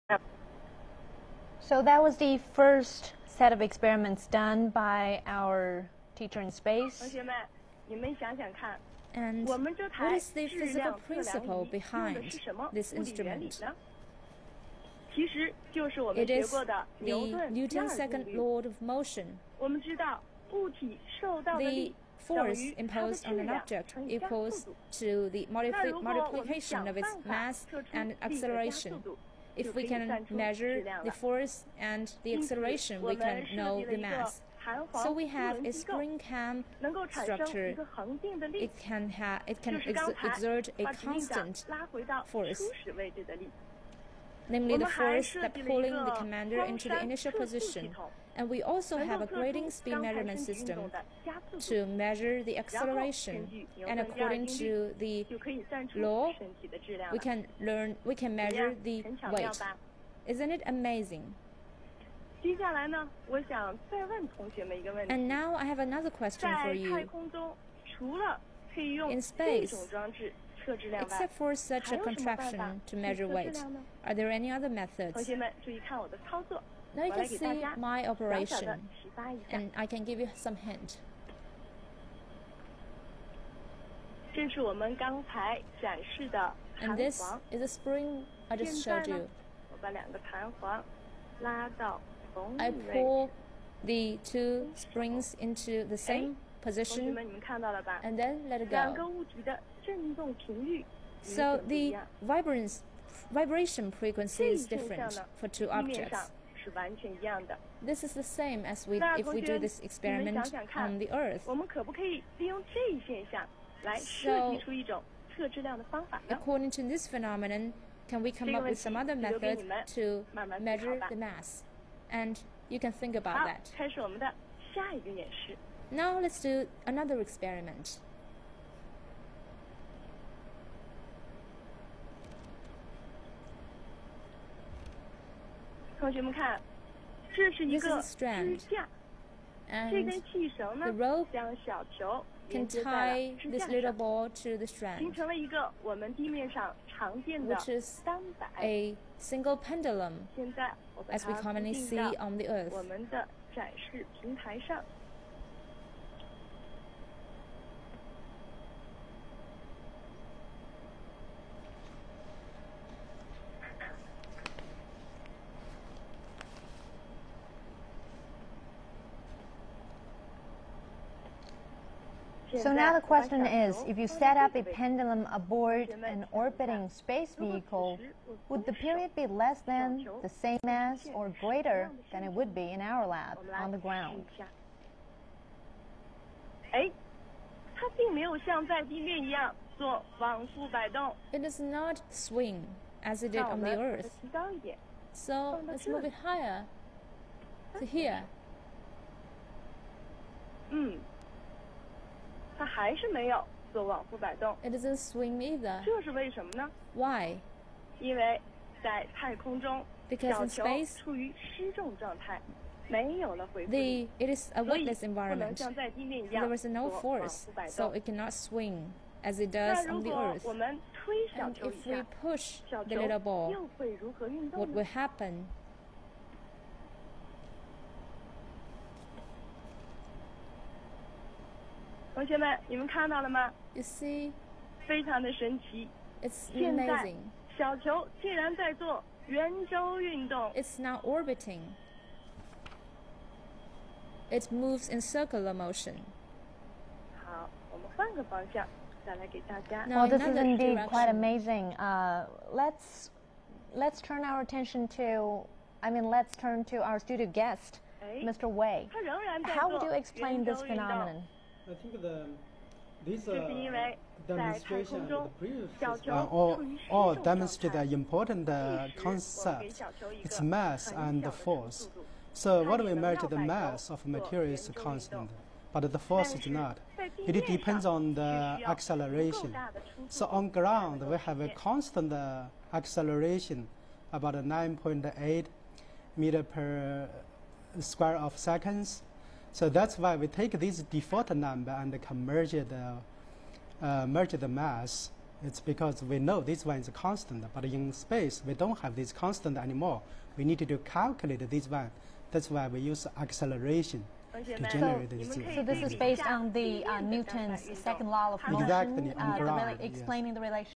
Experiment 2: demonstration of simple pendulum movement
Chinese astronaut Wang Yaping gives space lecture